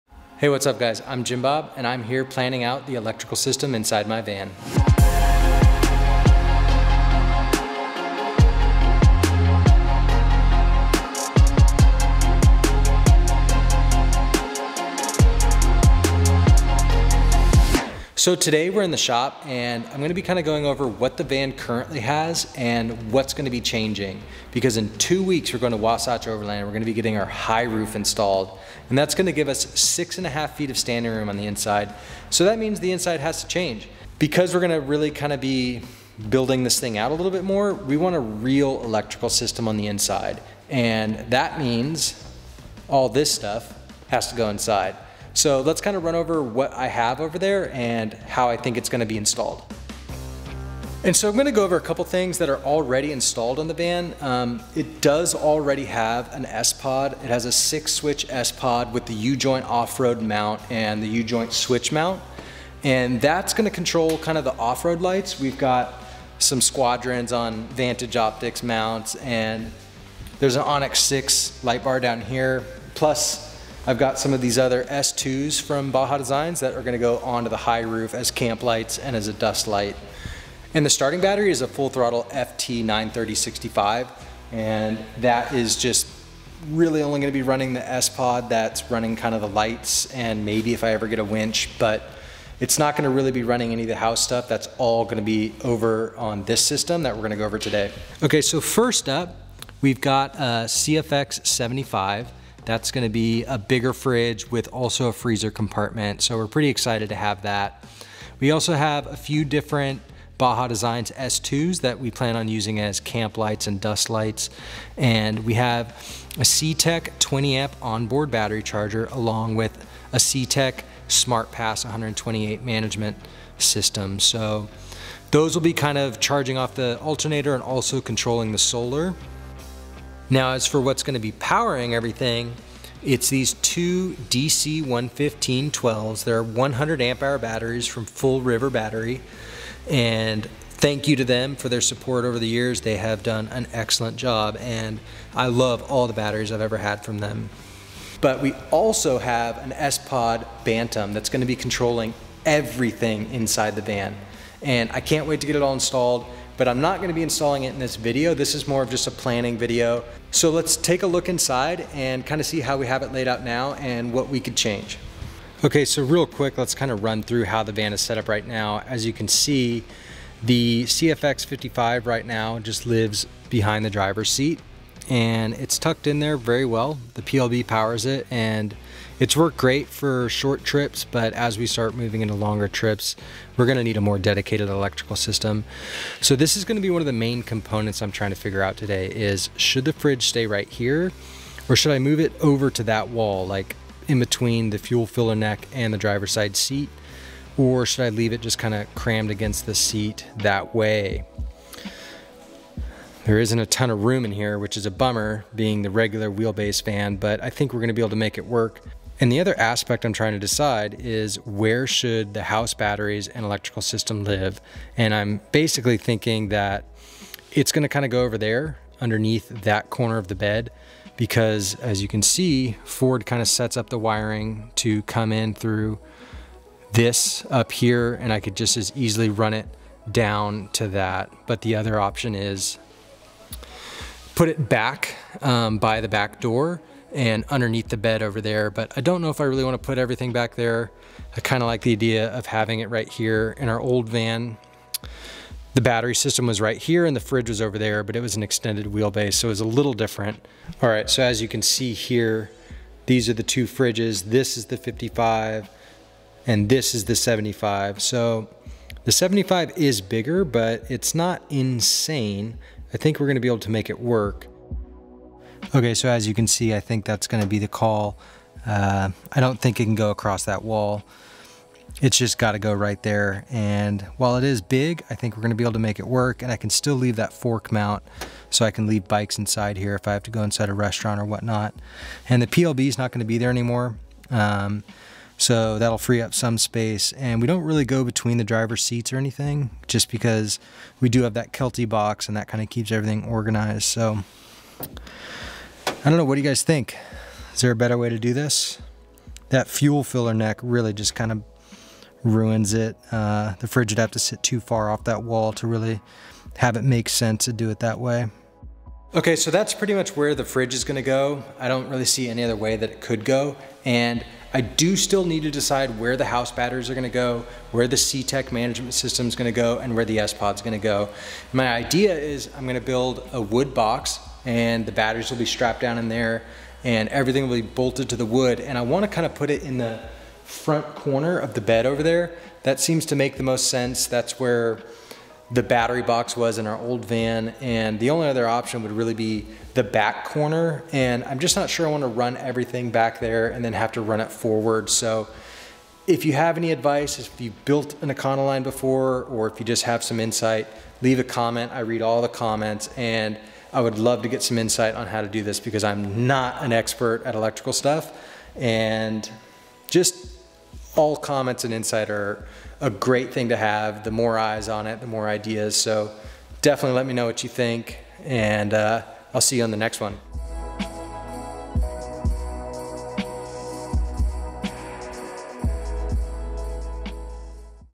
Complete overhaul on my Van life Battery and Electrical System!
Now that the suspension is done, it's time to focus on the house electrical system for the van. In this episode, I'm gonna go over what is already done to the van in terms of the electrical system and what is going to happen to it. We have a whole new house battery setup that needs to go in along with a bunch of other goodies, so follow along and give me some tips! I'd love to hear what you think about the layout and how I could improve it.

00:00 Intro
00:50 What's already installed
01:38 What's going in
02:45 The current interior setup
04:10 Comparing the old fridge to the new fridge
04:25 How the 75DZ fits inside
05:09 Outro